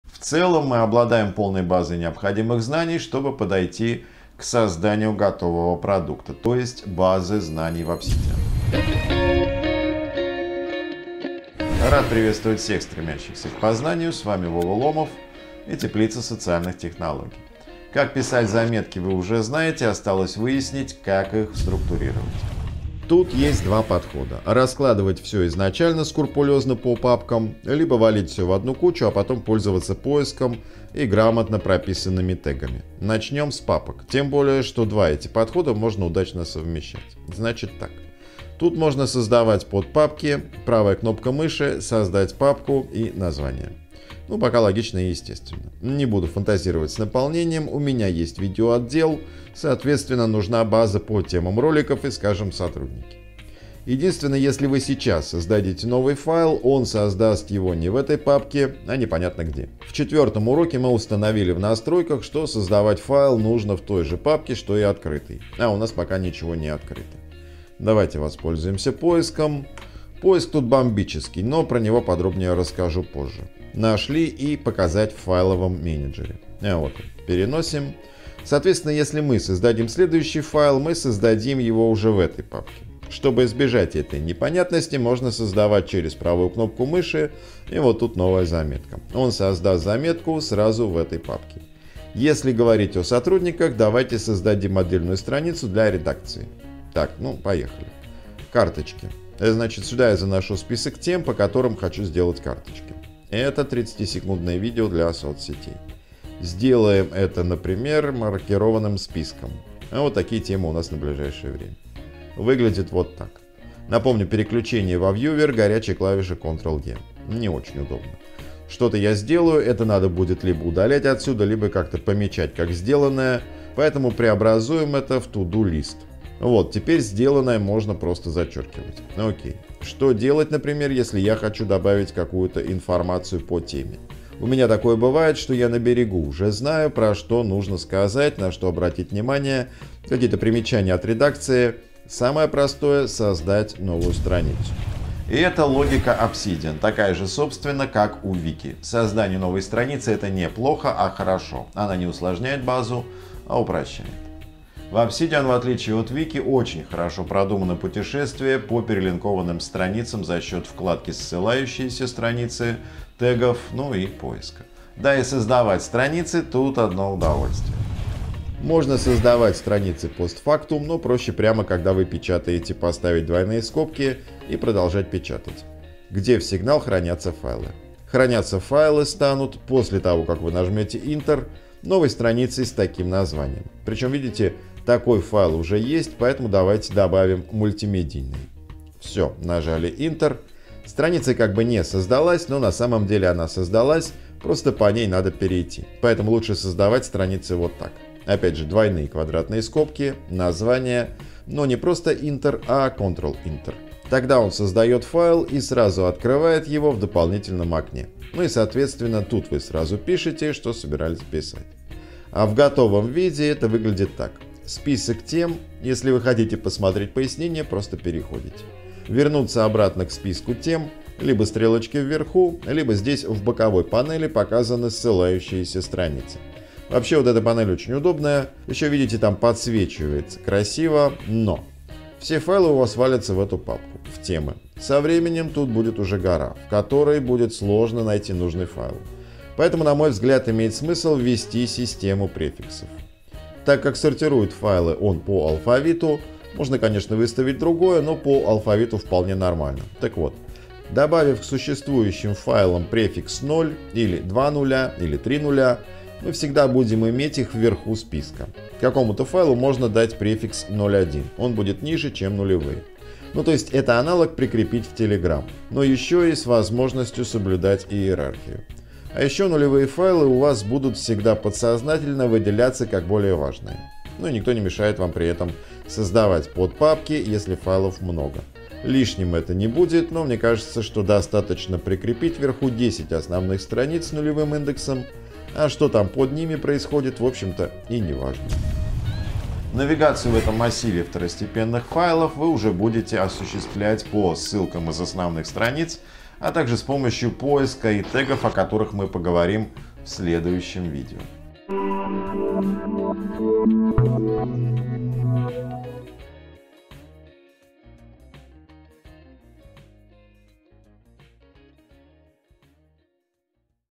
Obsidian уроки #6: файлы и папки – как лучше структурировать базу
Obsidian – приложение для работы с базами знаний. А что такое база знаний? По сути это мозг, который структурирует весь объем полученной вами информации. Примерно тем же занимается и Obsidian.

Формально Obsidian – это оболочка для работы с файлами Markdown и это главное достоинство этой программы. Markdown – облегченный язык разметки документов, который не связан с какой-то одной программой или приложением, а значит открывать эти документы вы можете где угодно, просто в Obsidian с ними работать удобнее, чем в других программах.

Таким образом в отличие от проприетарного ПО в случае с Obsidian вы не связаны с конкретным разработчиком. Хранить базу вы можете где угодно и что бы не случилось в стране и мире, вы ее не потеряете, ну разве что по собственной вине.